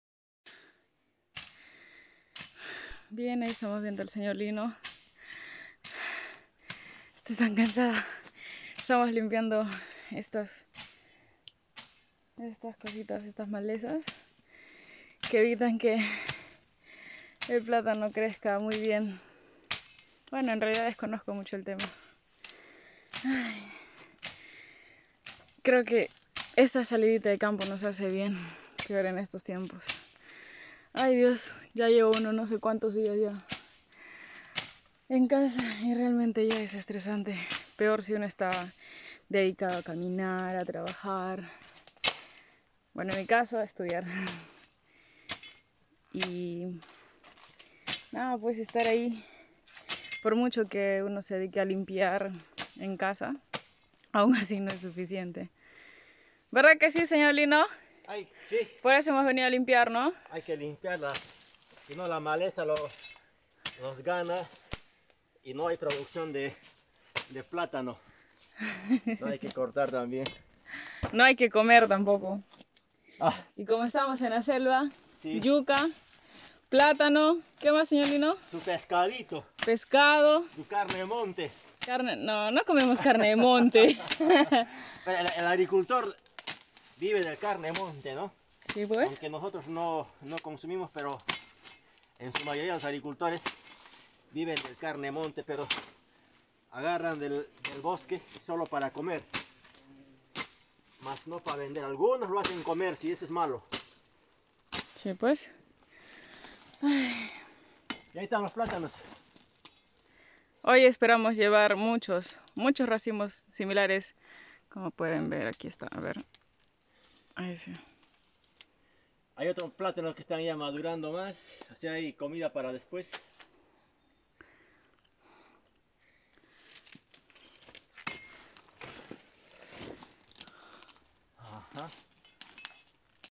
CULTIVO DE CHACRA DE PLATANOS
Es muy barato pagar por un kilo de plátanos.. Pero es mucho la tarea de cuidar y cultivar para que las mala yerbas no las asfixie ... También existen ladrones que sin trabajar se roban el sacrificio de los Agricultores  Amazónicos... Este trabajo es muy pesado en el clima tropical, terminé con las manos adormecidas y el antebrazo adolorido... Todo es vivir para contarlo... Valoremos a los Dignos Agricultores Peruanos.